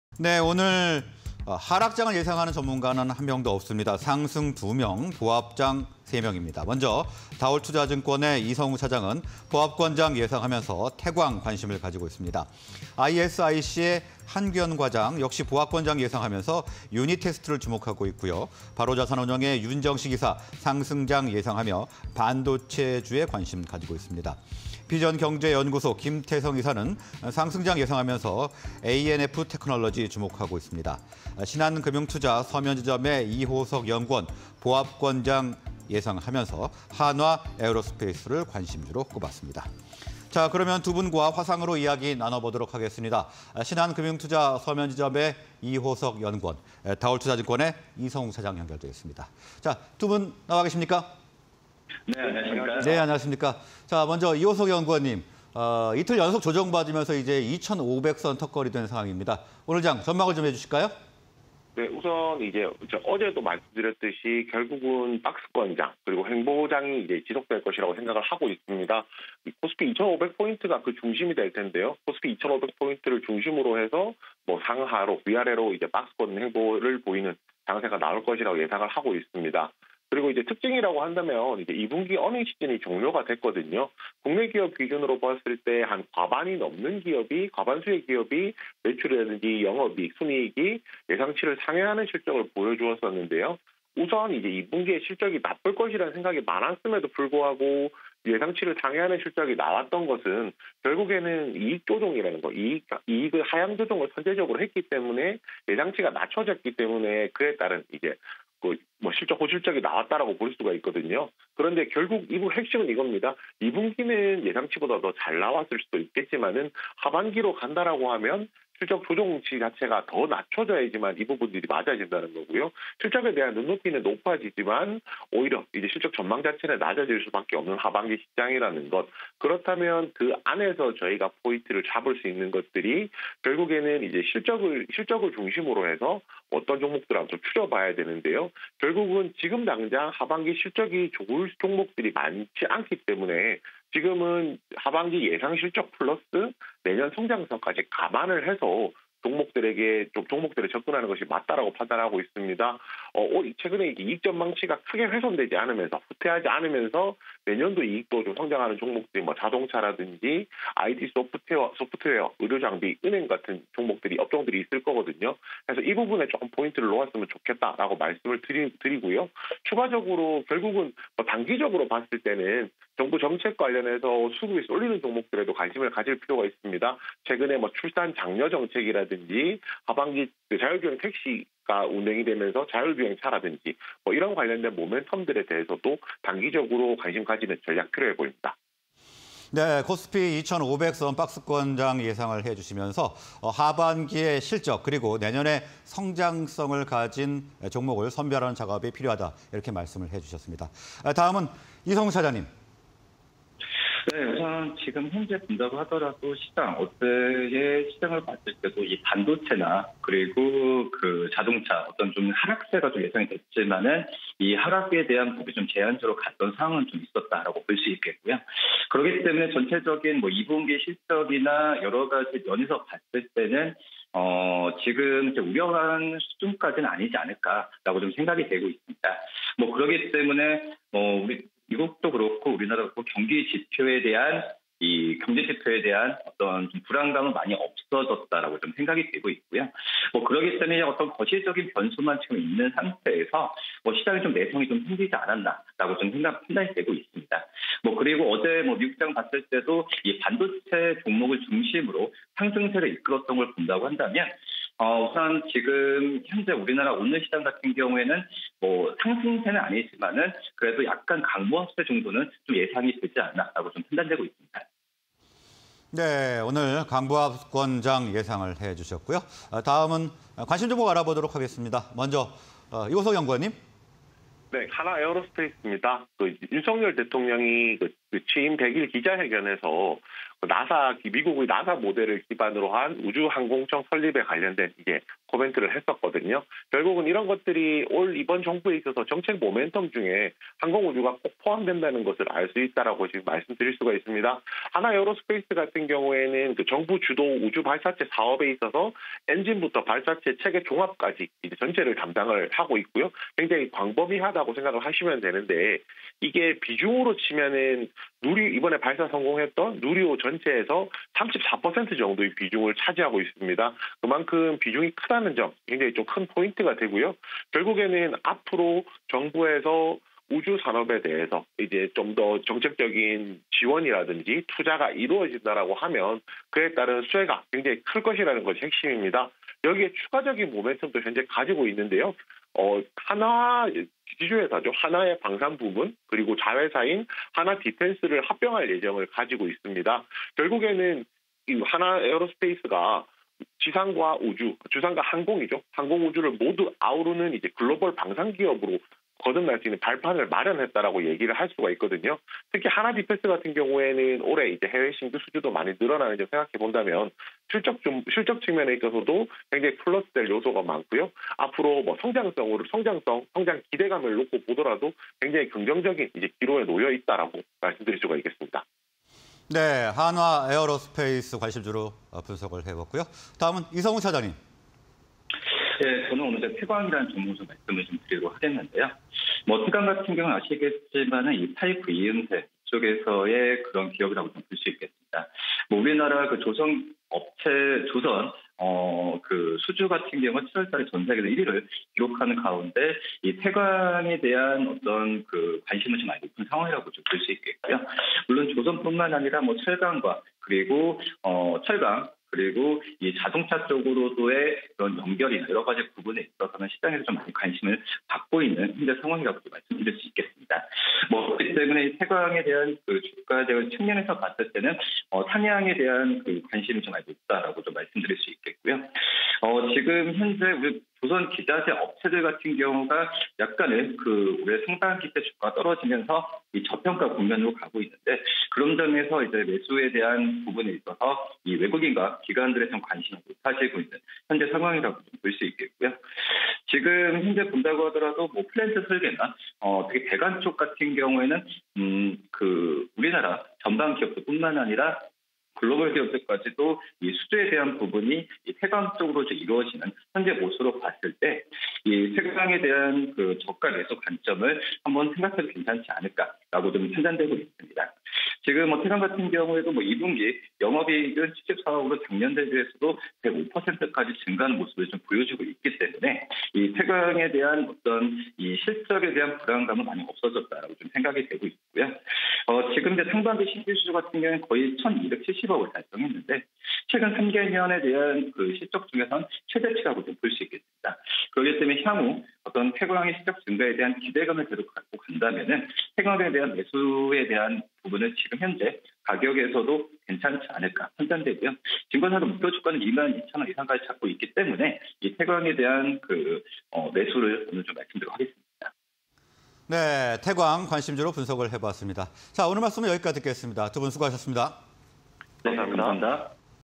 태광 등 기계 업종 / 한화에어로스페이스... 尹정부 우주항공청 설립 발표_UP&DOWN 5인의 투자 전략 (20220819)
Q. 코스피, 2500선 턱걸이… 하반기 실적 전망 '암울'?
- 코스피 2,500선 중심의 박스권 횡보 전망
- 2분기 어닝시즌 종료, 과반수 이상 실적 예상치 상회
- 높아진 실적 눈높이 VS 낮아진 하반기 실적 전망
- 내년 성장성 감안한 실적 모멘텀 보유 종목 접근
- 정책적 모멘텀 보유 업종 단기 관심

- 尹정부 우주항공청 설립 발표
- 정부 주도 우주발사체 사업 주목
- 누리호 전체 중 34% 부품 제조
- 한화디펜스와 한화 방산부문 합병 예정

Q. 2,500선서 숨고르는 증시… "더 오른다" VS "떨어진다"?
- 美 경제지표 '혼조세'… 반도체주 상승 주목
- 태광 등 기계 업종 관심